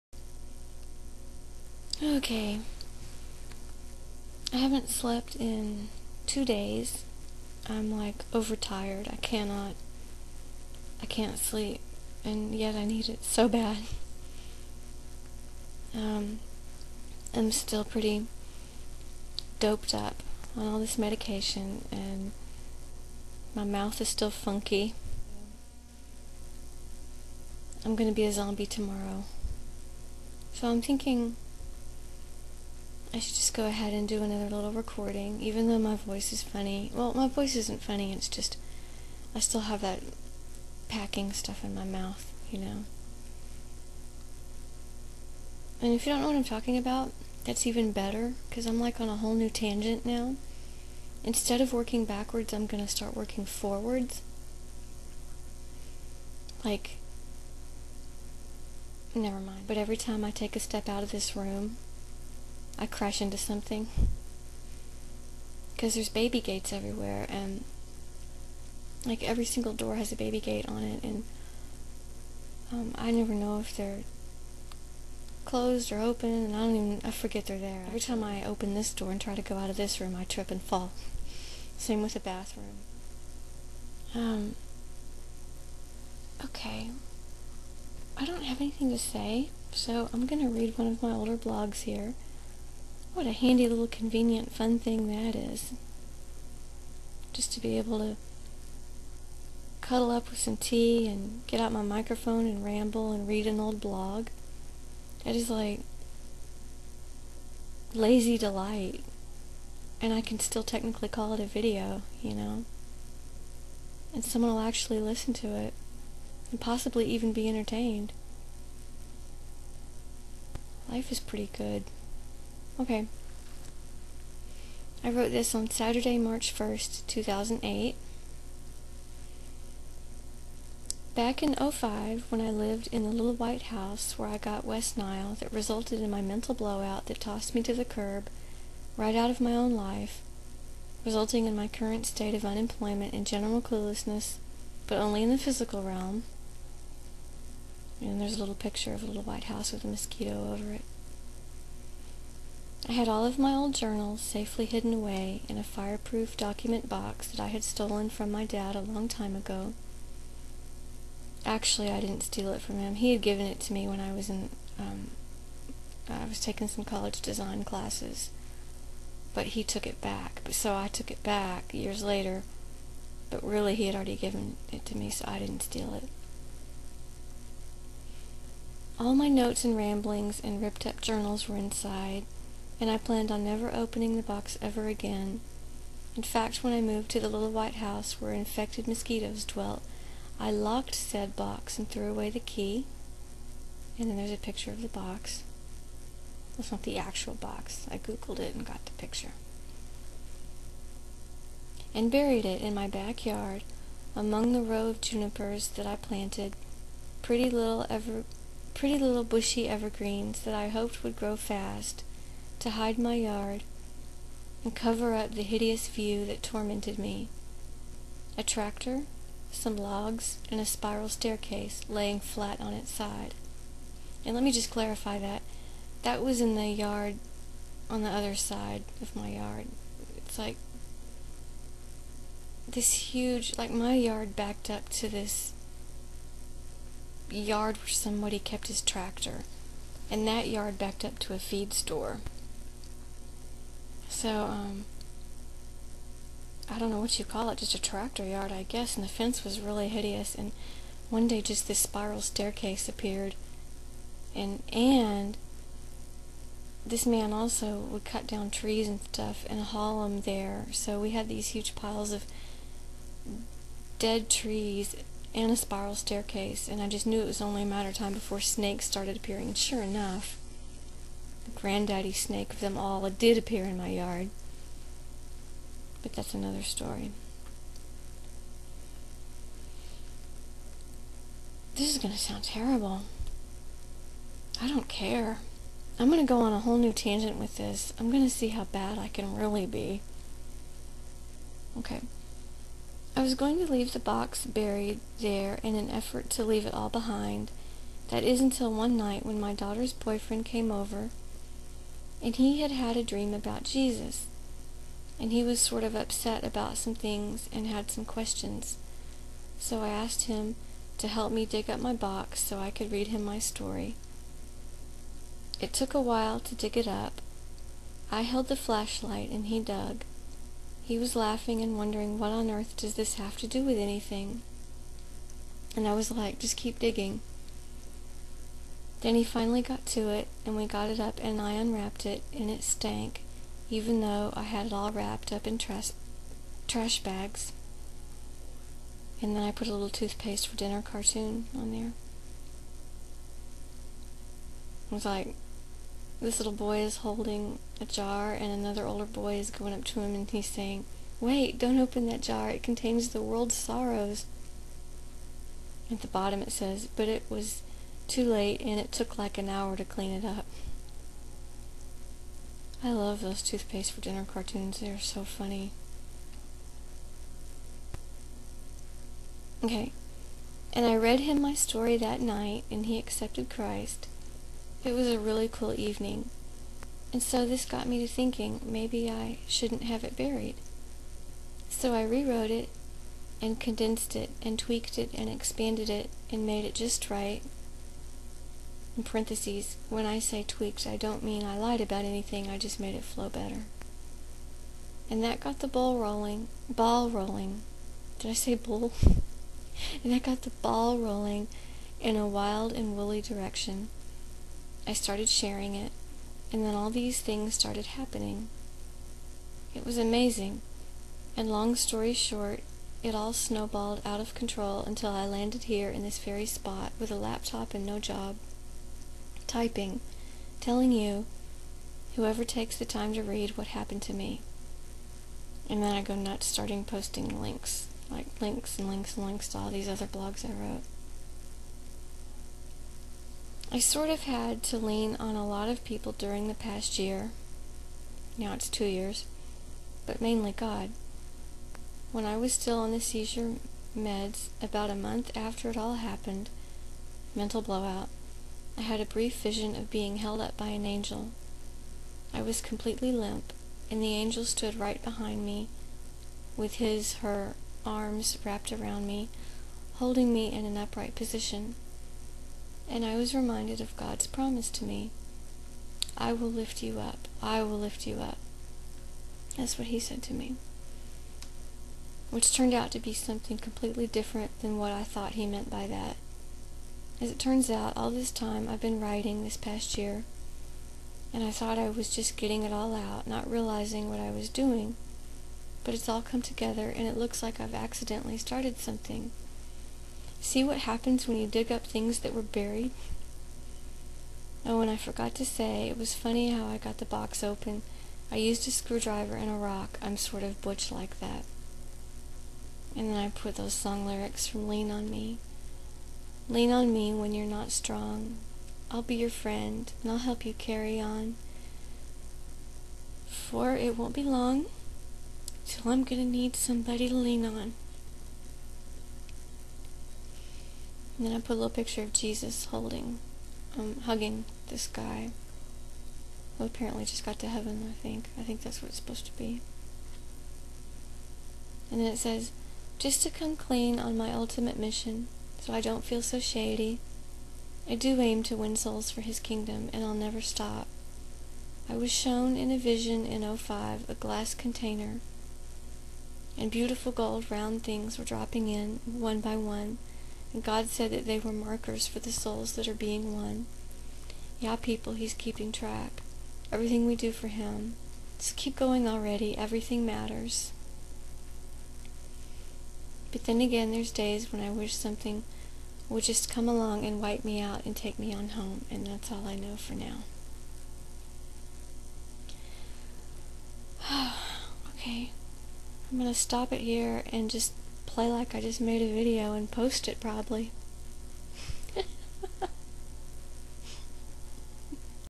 chamomile tea
sipping tea and reading off an old blog. what more do you want